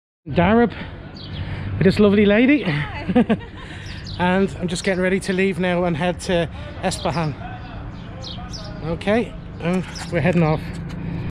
Darab to estabhan on a Himalayan (1)
my trip through biran on my Himalayan